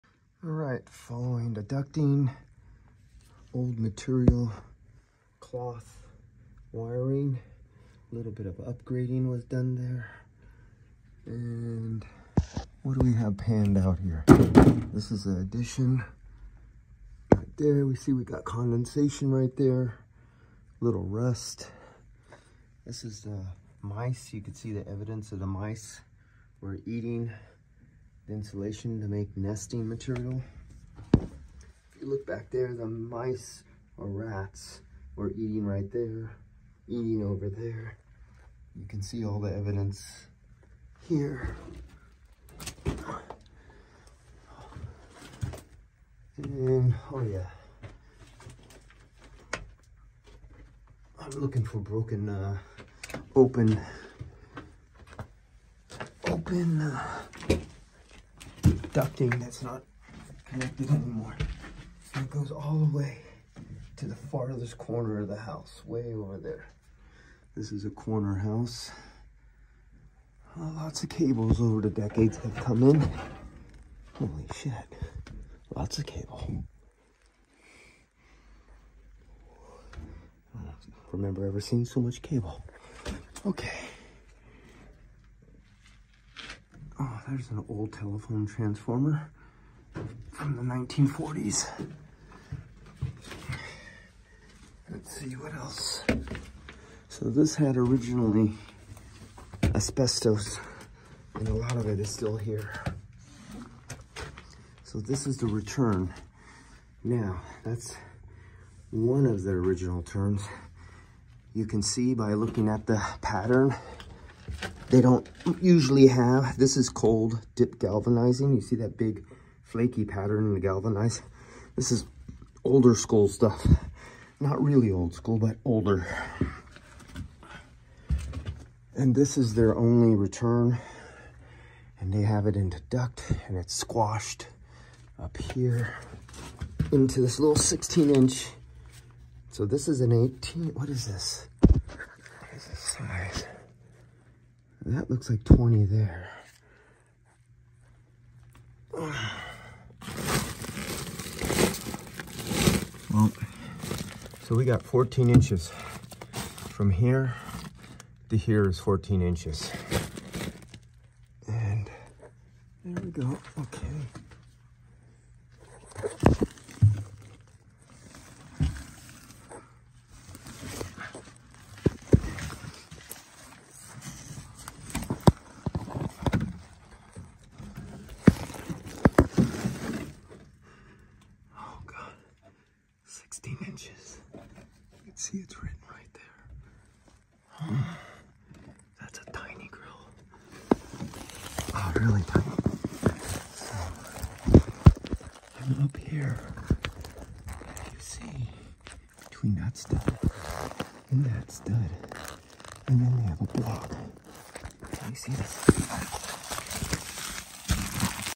Rat infested, air ducting, extremely undersized return squashed, and folded.
Kinked 90° 16 inch or 14 inch return ducting on 110,000 BTU furnace 5 ton AC system. About 30 feet long..
      A lot of air ￼restriction.